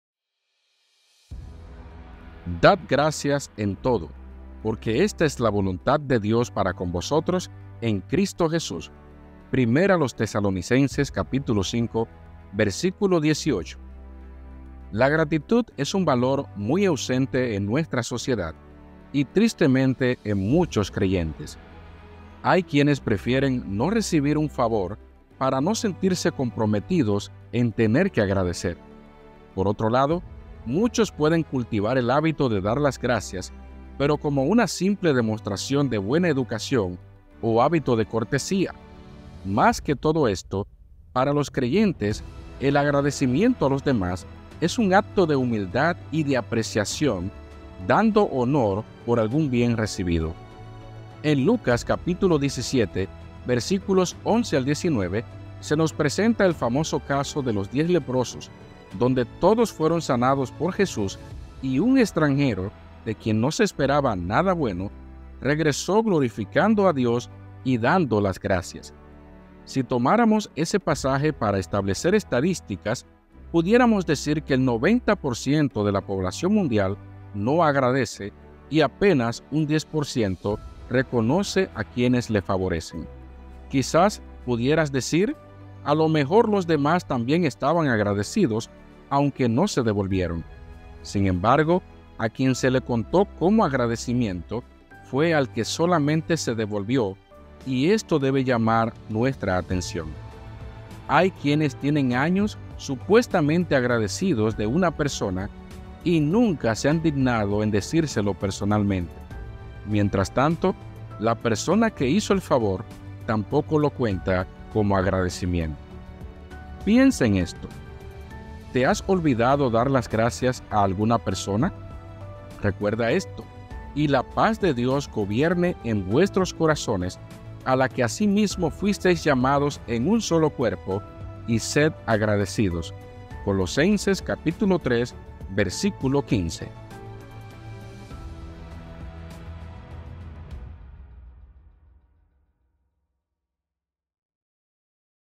REFLEXIÓN DEL DÍA | Agradecer a los Demás - Smaily Rosario
REFLEXIÓN DEL DÍA
«Dad gracias en todo, porque esta es la voluntad de Dios para con vosotros en Cristo Jesús (1 Taselonicenses 5:18).

La gratitud es un valor muy ausente en nuestra sociedad, y tristemente en muchos creyentes. Hay quienes prefieren no recibir un favor, para no sentirse comprometidos en tener que agradecer. Por otro lado, muchos pueden cultivar el hábito de dar las gracias, pero como una simple demostración de buena educación o hábito de cortesía. Más que todo esto, para los creyentes el agradecimiento a los demás es un acto de humildad y de apreciación; dando honor por algún bien recibido.

En Lucas 17:11-19 se nos presenta el famoso caso de los diez leprosos, donde todos fueron sanados por Jesús y un extranjero (de quien no se esperaba nada bueno), regresó glorificando a Dios y dando las gracias. Si tomáramos ese pasaje para establecer estadísticas, pudiéramos decir que el 90% de la población mundial no agradece, y apenas un 10% reconoce a quienes le favorecen. Quizás pudieras decir: «A lo mejor los demás también estaban agradecidos, aunque no se devolvieron». Sin embargo, a quien se le contó como agradecimiento fue al que solamente se devolvió, y esto debe llamar nuestra atención. Hay quienes tienen años supuestamente agradecidos de una persona, y nunca se han dignado en decírselo personalmente; mientras tanto, la persona que hizo el favor tampoco lo cuenta como agradecimiento.

PIENSA EN ESTO
¿Te has olvidado dar las gracias a alguna persona?

RECUERDA ESTO
«Y la paz de Dios gobierne en vuestros corazones, a la que asimismo fuisteis llamados en un solo cuerpo; y sed agradecidos» (Colosenses 3:15)

_Smaily Rosario_